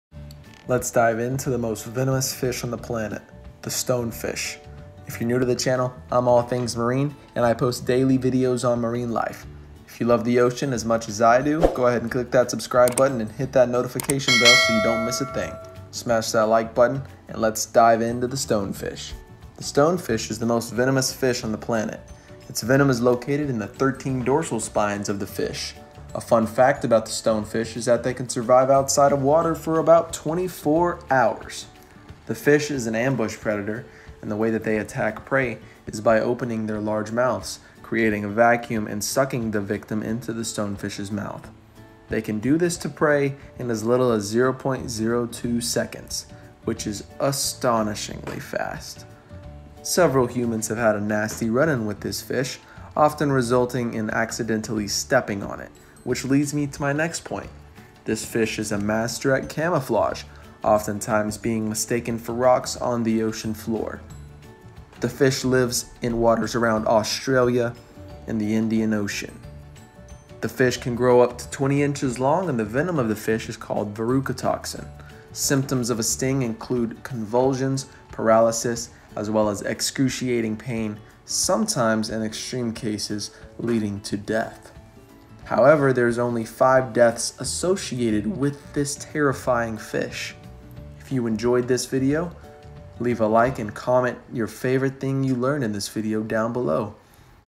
Most Venomous FISH On The Planet!...STONEFISH
Just be happy and Enjoy...

Lets dive in to the stonefish. The stonefish is the most venomous fish on the planet. Its venom is located in the 13 dorsal spines of the fish. A fun fact about the stonefish is that they can survive outside of the water for almost 24 hours.
Animals and Insect TV
License